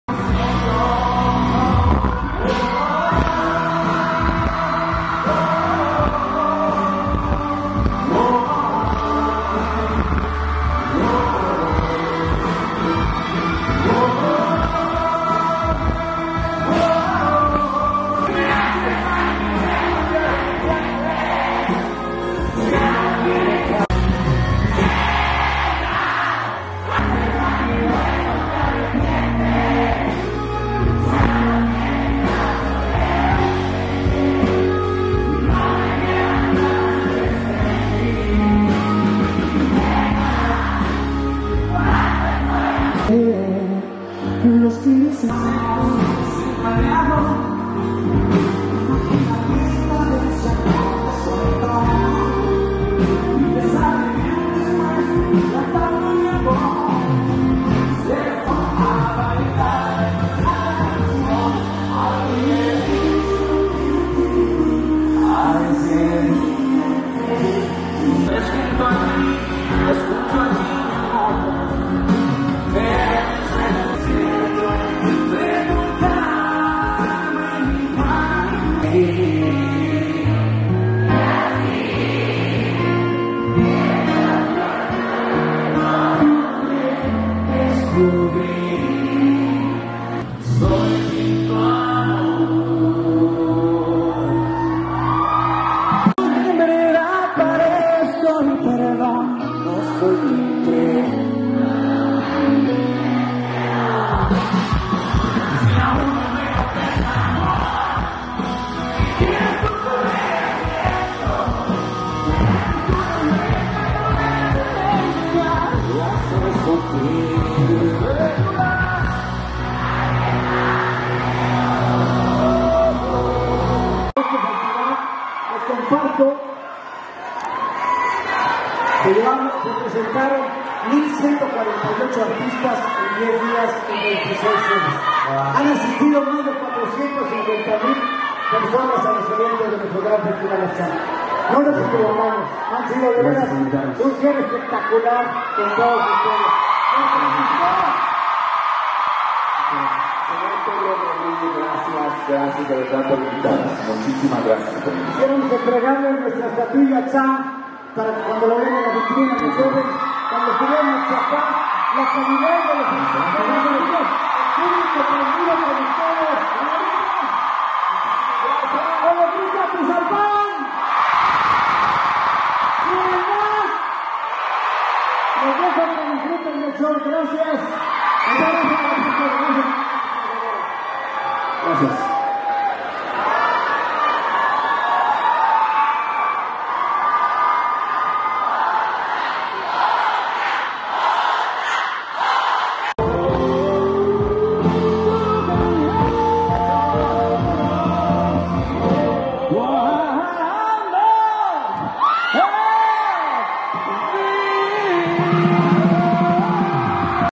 Camila en Atizapán de Zaragoza Festival Internacional Atzán
Camila encendió al público de Atizapán de Zaragoza y lo hizo cantar en una sola voz con sus canciones en Festival Internacional Atzán de @GobAtizapan En total, en los eventos de más de una semana participaron 1,480 artistas que convocaron a más de 450 mil personas PedroRodriguez